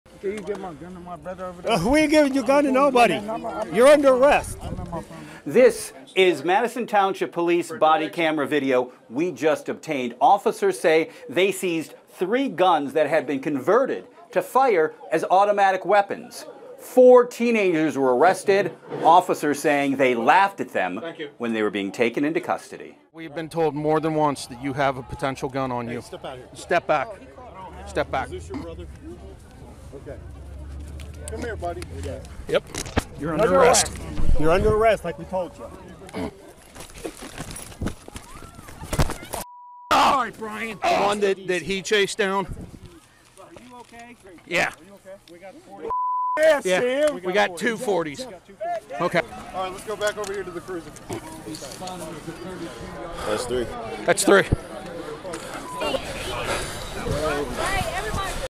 Guns seized by police at Ohio high school football game
Madison Township Police are working with nearby school districts to make football games safer after three weapons were seized during a Friday night high school game in Canal Winchester.